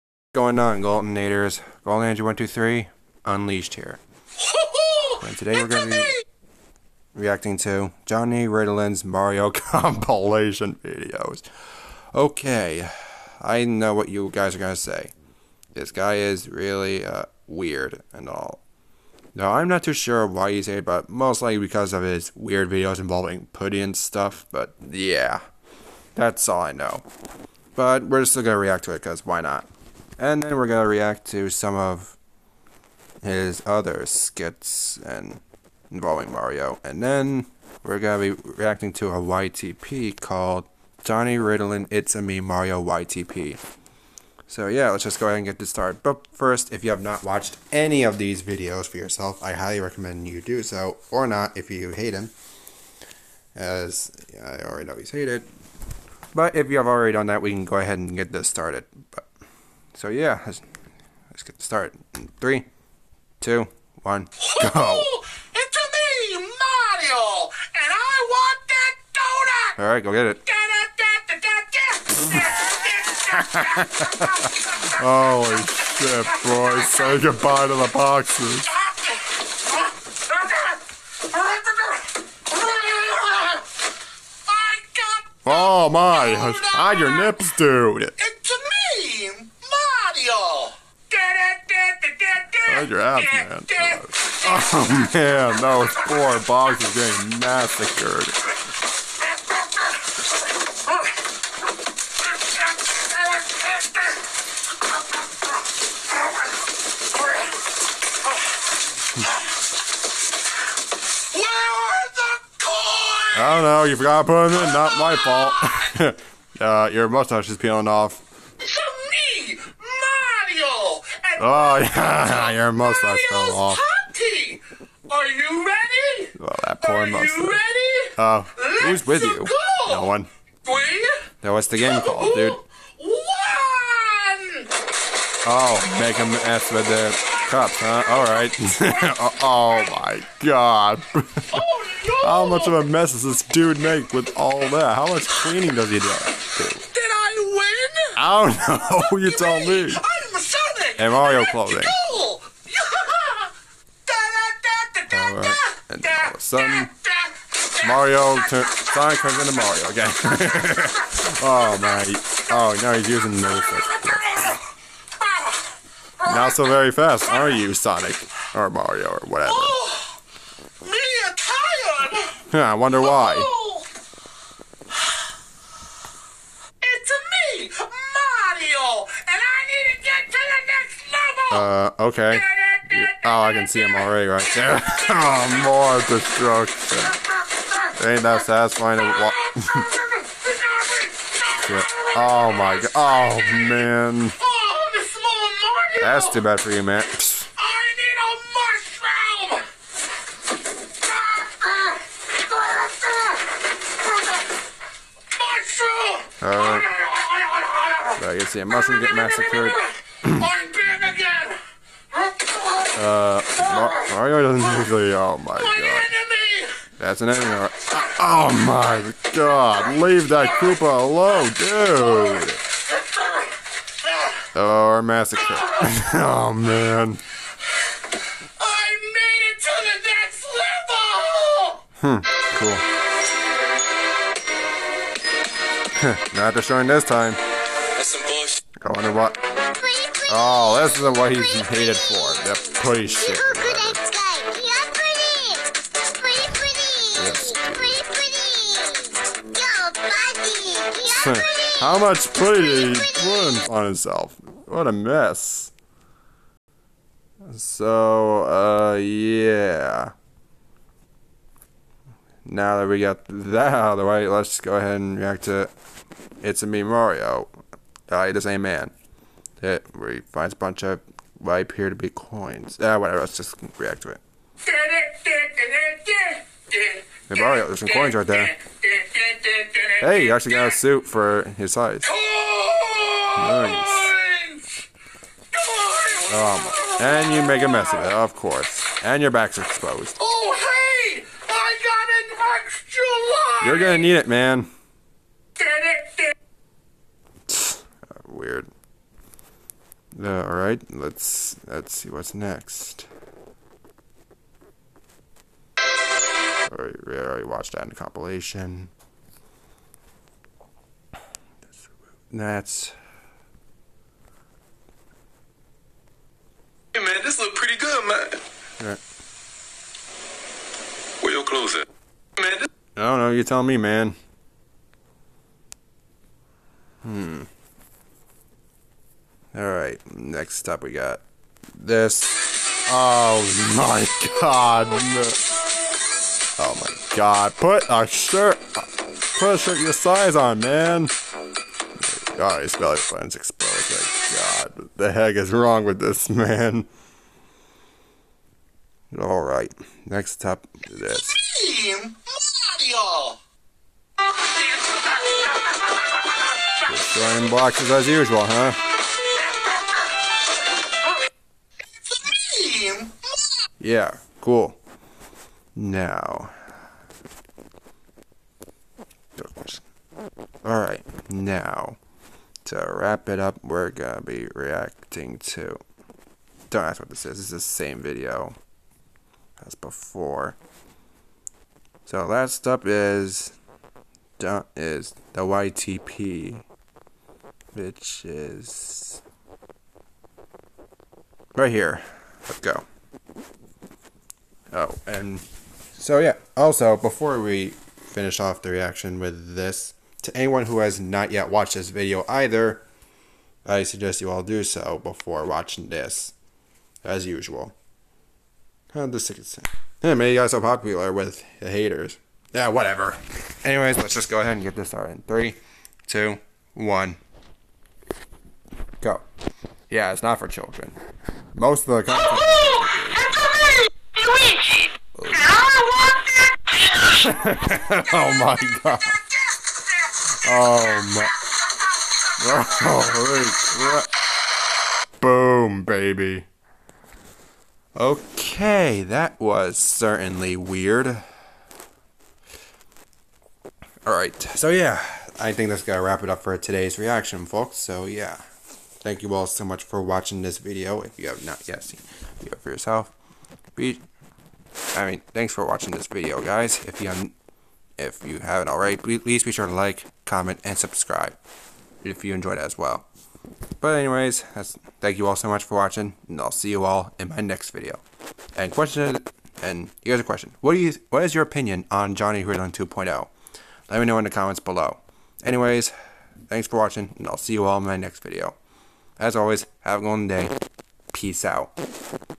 Reacting to JohnniRiddlin2.0's Mario Compilation and Shorts + JohnniRiddlin2.0 it's a me YTP
Credit to @Jonnyriddlin1 for the creation of these Shorts and the Compilation
Credit to @adianlmao for uploading the Short YTP